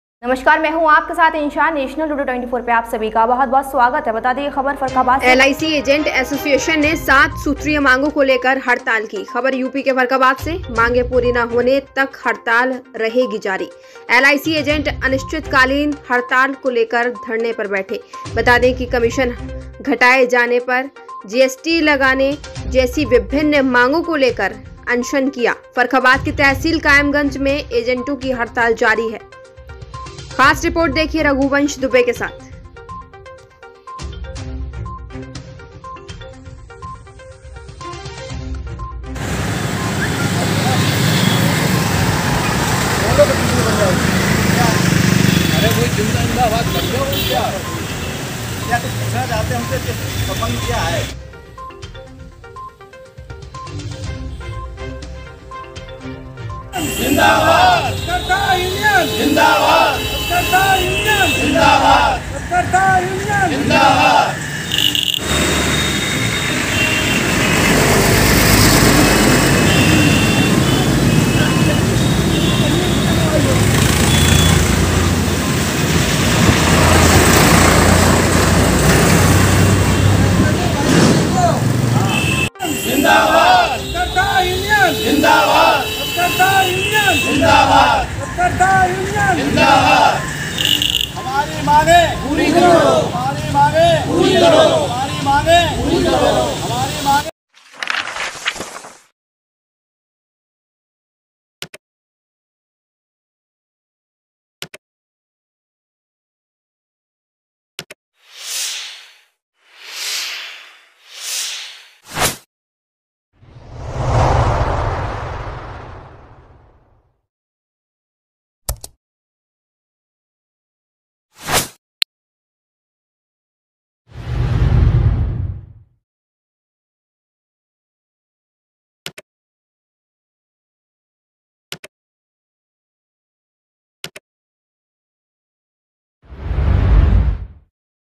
farrukhabad मांगे पूरी ना होने तक हड़ताल रहेगी जारी
फर्रुखाबाद-एलआईसी एजेंट एसोसिएशन ने 7 सूत्री मांगों को लेकर की हड़ताल।